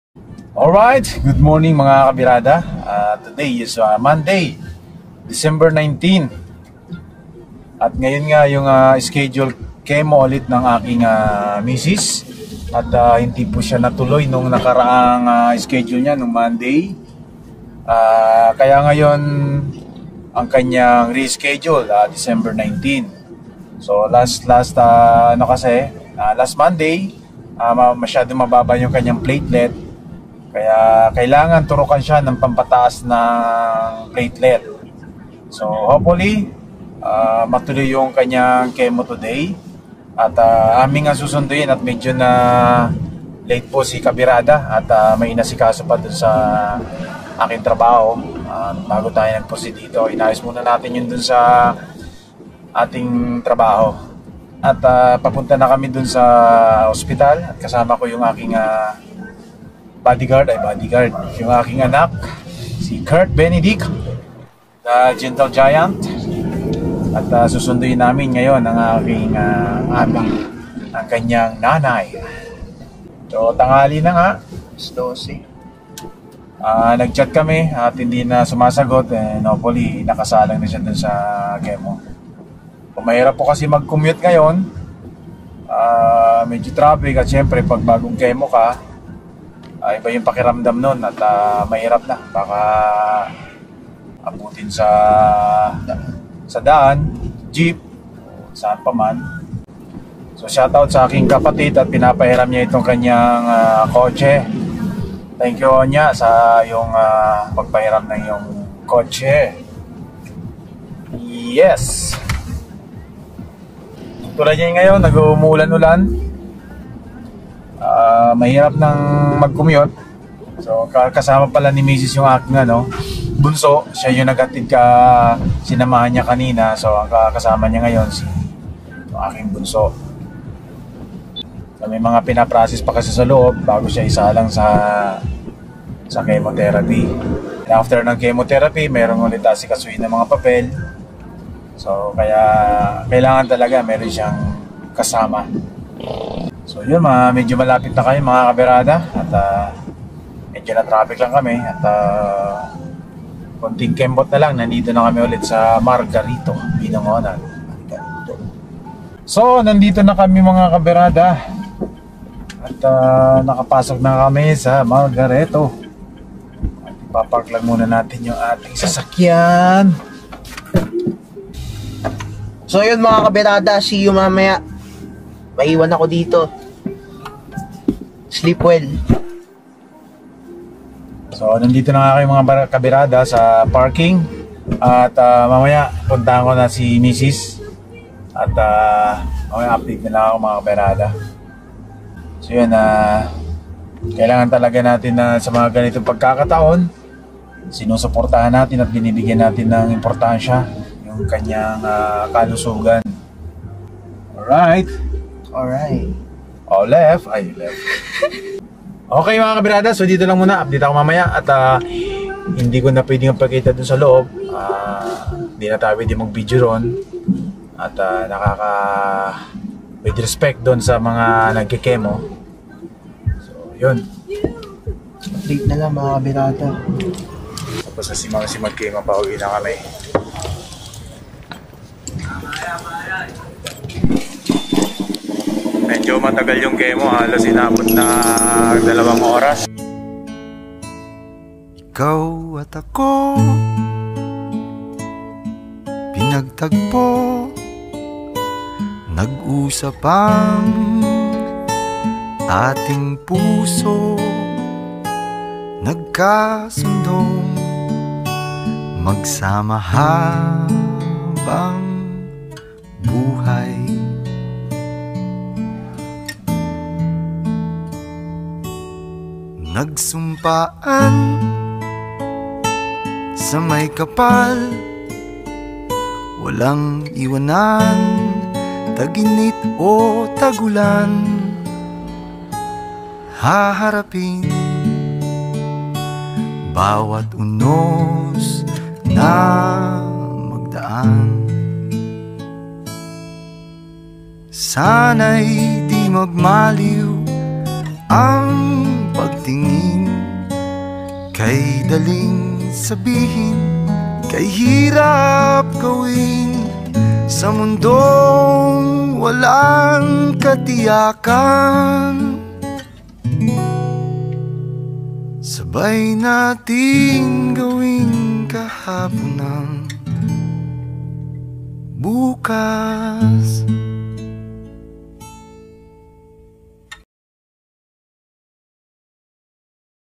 EP. 3 SECOND SESSION CHEMOTHERAPY SERYE
Ang Bilis ng epekto ng gamot nahilo at nagsusuka agad sya..